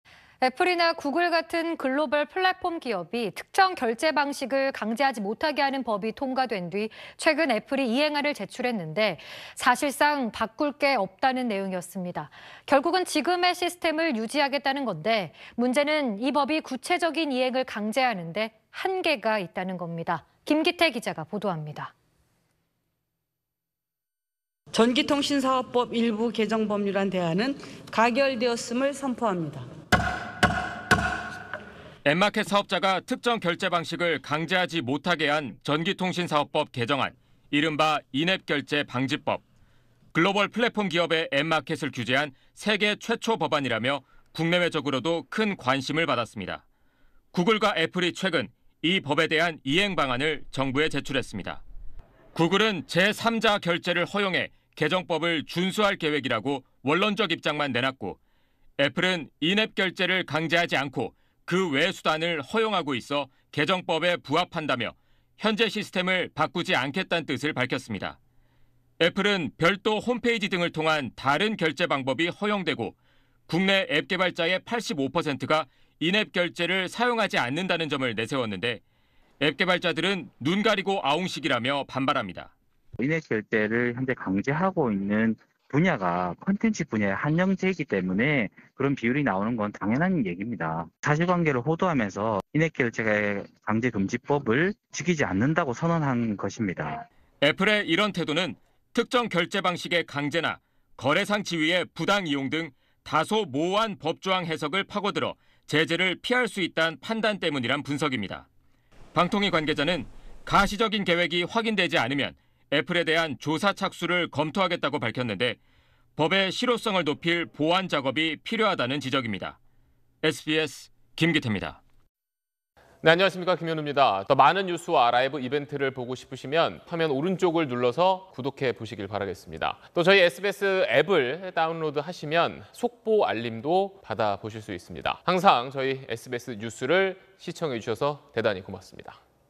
'인앱결제 금지' 무시하는 애플…맹탕 이행안 제출 / SBS
애플이나 구글 같은 글로벌 플랫폼 기업이 특정 결제방식을 강제하지 못하게 하는 법이 통과된 뒤 최근 애플이 이행안을 제출했는데, 사실상 바꿀 게 없다는 내용이었습니다. 결국은 지금의 시스템을 유지하겠다는 건데, 문제는 이 법이 구체적인 이행을 강제하는 데 한계가 있다는 겁니다.
김기태 기자가 보도합니다.